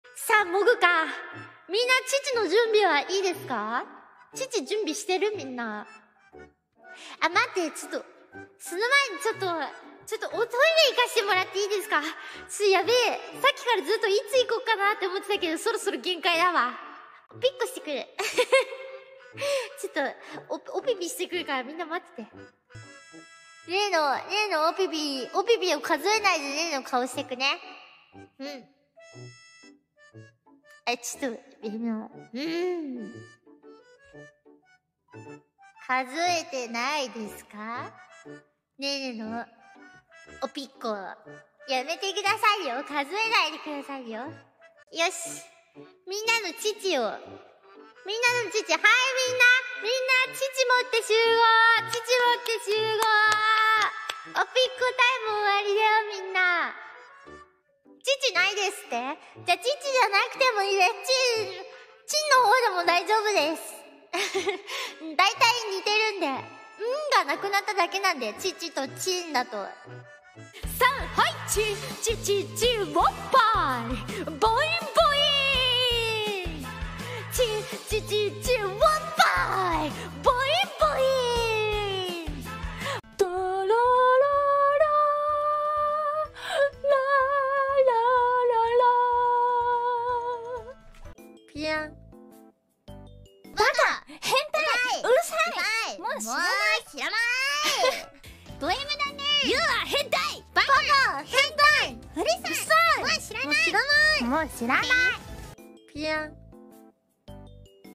Nene要大家一起來甩奶..沒有奶就甩GG吧! 反正發音差不多ww【hololive中文】【Vtuber精華】【桃鈴ねね】【MomosuzuNene】【桃鈴音音】
【 歌枠 】まったり雑談歌枠…♡【 桃鈴ねね / ホロライブ 】